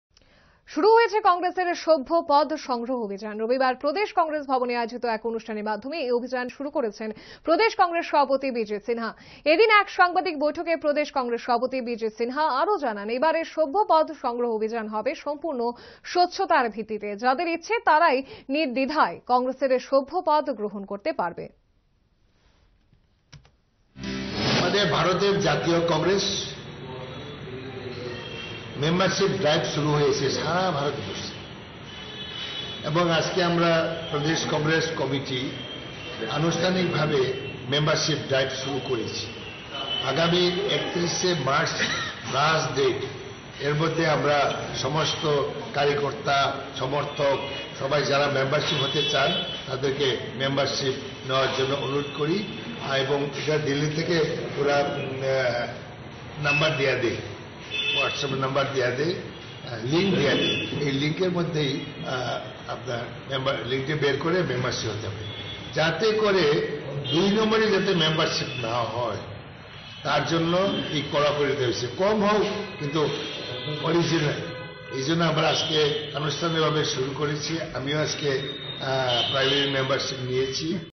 প্রদেশ কংগ্রেস ভবনে শুরু হয়েছে কংগ্রেসের সভ্য পদ সংগ্রহ অভিযান | Congress | Tripura News | Times24
সম্পূর্ণ স্বচ্ছতার ভিত্তিতে প্রদেশ কংগ্রেস ভবনে শুরু হয়েছে কংগ্রেসের সভ্য পদ সংগ্রহ অভিযান | State Level Training Camp